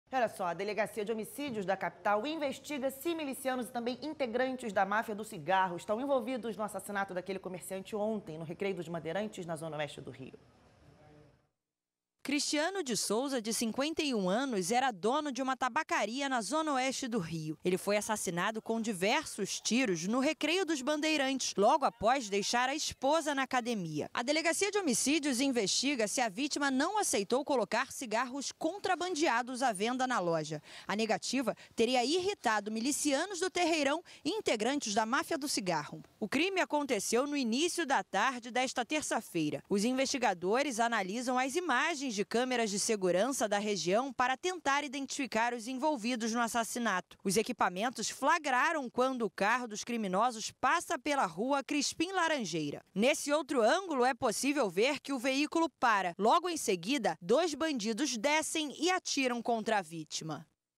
Polícia investiga assassinato no Recreio
A delegacia de homicídios da capital investiga se milicianos e integrantes da máfia do cigarro estão envolvidos no assassinato de um comerciante no Recreio dos Bandeirantes.